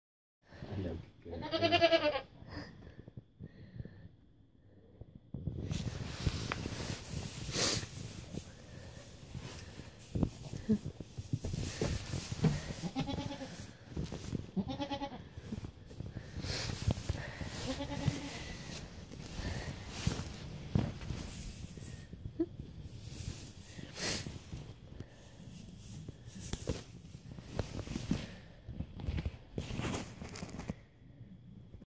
Goats in sweaters
I don't think they like their new look